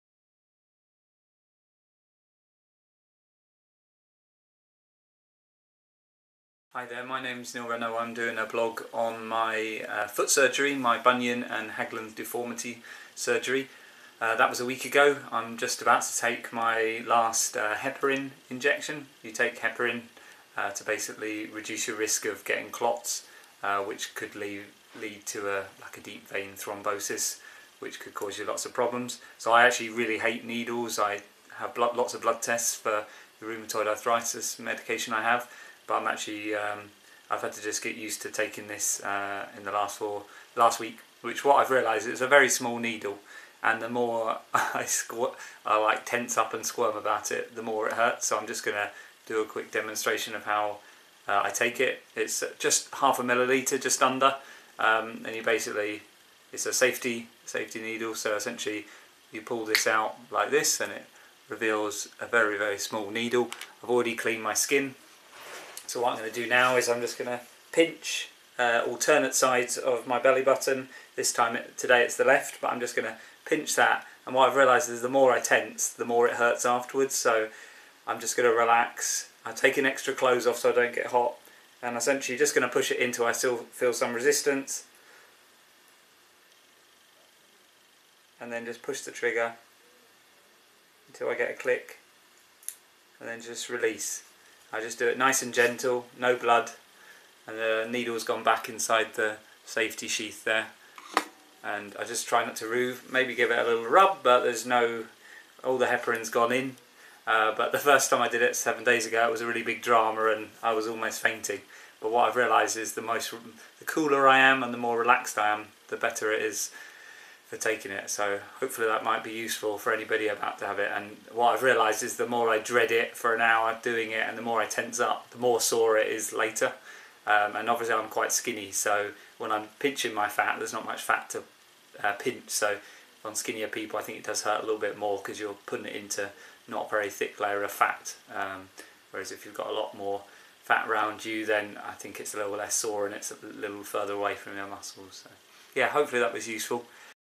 Post #5 - Day 7 - Taking Heparin injections for foot surgery
Day 7 post of Neil Renault's blog about a runner having bunion & haglund's deformity surgery.